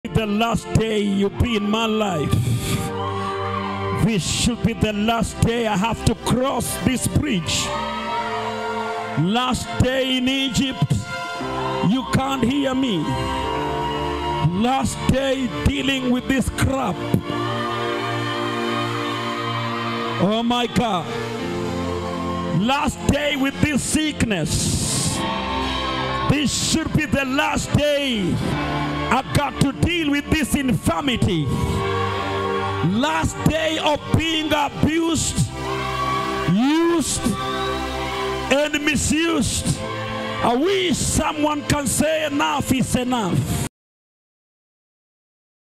THIS IS THE LAST DAY YOU WILL BE IN MY LIFE
THIS IS THE LAST DAY YOU WILL BE   IN MY LIFE,THE LAST DAY I HAVE TO BE CROSSING THIS BRIDGE..THE LAST DAY IMDEALING WITH THIS INFANITY. THE LAST DAY WITH THIS SICKNESS.

MINISTERING : REVIVALIST L.A SHADRACKS

SERMON |THIS SHOULD BE THE LAST DAYS, ENOUGH IS ENOUGH |